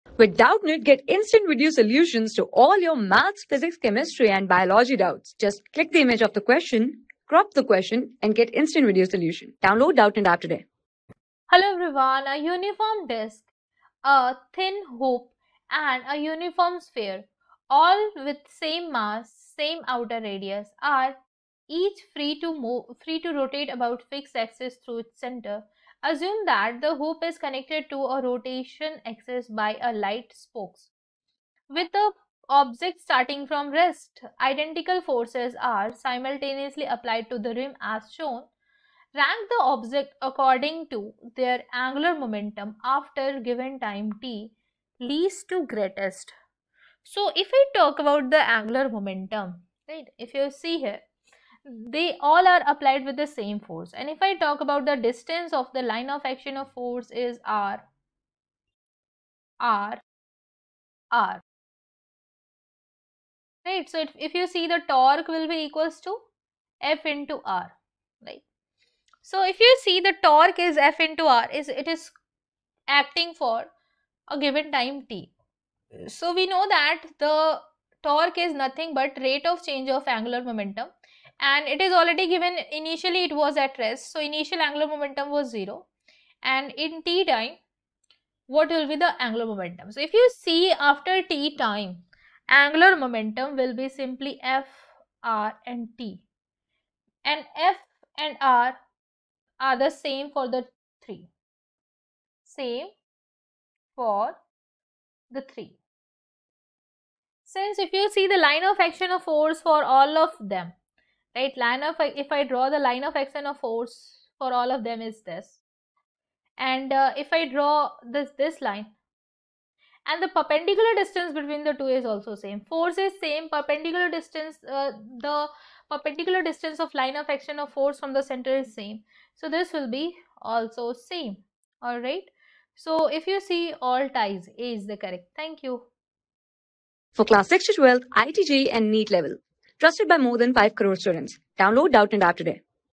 A uniform disk, a thin hoop (ring), and a uniform sphere, all with the same mass and same outer ...
A uniform disk, a thin hoop (ring), and a uniform sphere, all with the same mass and same outer radius, are each free to rotate about a fixed axis through its center. Assume the hoop is connected to the rotation axis by light spokes. With the objects starting from rest, identical forces are simultaneously applied to the rims, as shown. Rank the objects according to their angular momentum after a given time t, least to greatest.
Class: 12
Subject: PHYSICS
Chapter: TEST PAPERS
Board:IIT JEE

👉 Have Any Query? Ask Us.